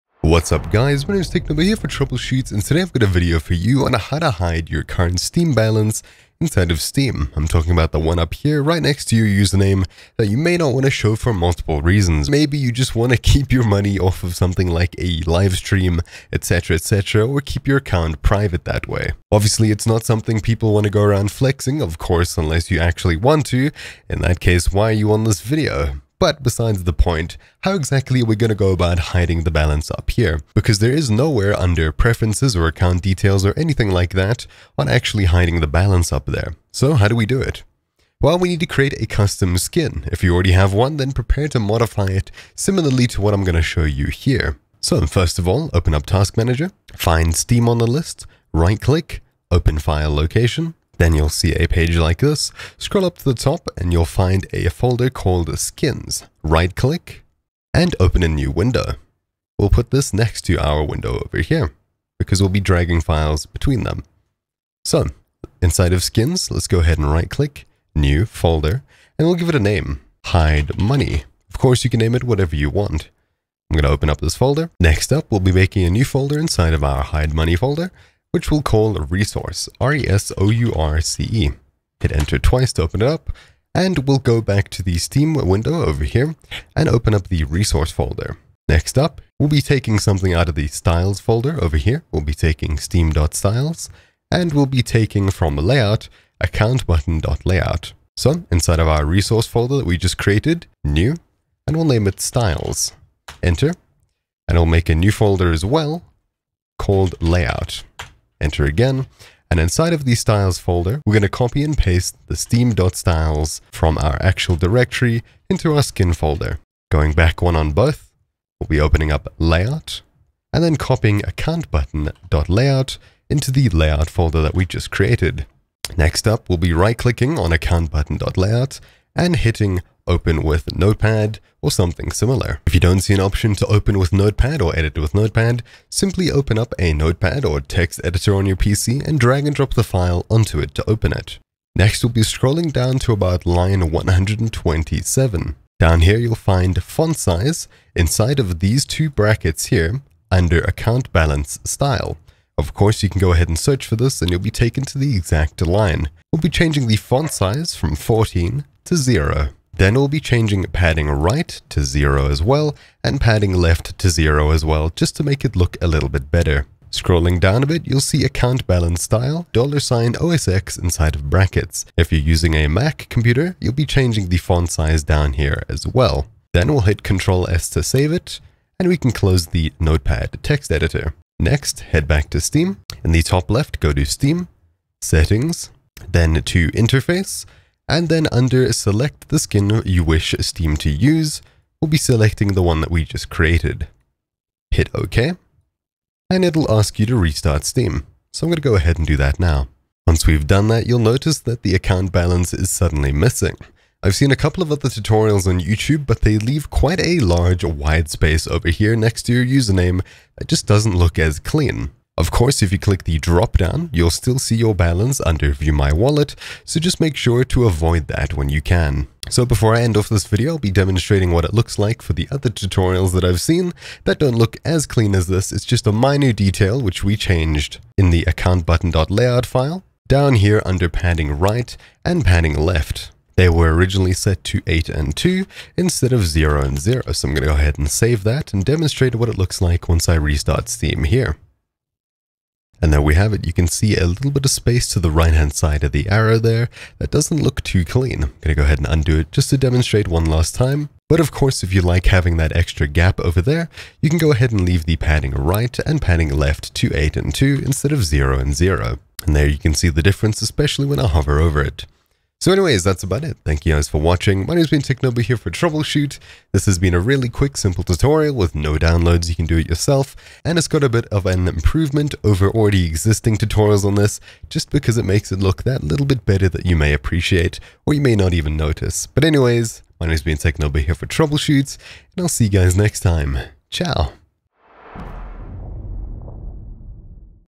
Hide Steam Wallet Balance | Useful for streamers & creators
If you create content and you would rather keep your Steam Account balance private, then you need a way to hide it. Of course, placing it on another screen can still be shown if something even so much as 'glitches out' on the live stream. So, instead of waiting for it to accidentally be broadcast to the internet, you can hide it completely. (Of course, you can still use your wallet balance)

Timestamps
0:00 - Explanation
0:37 - Locating files
1:04 - Creating a new skin
2:12 - Hiding balance
3:03 - Select skin in Steam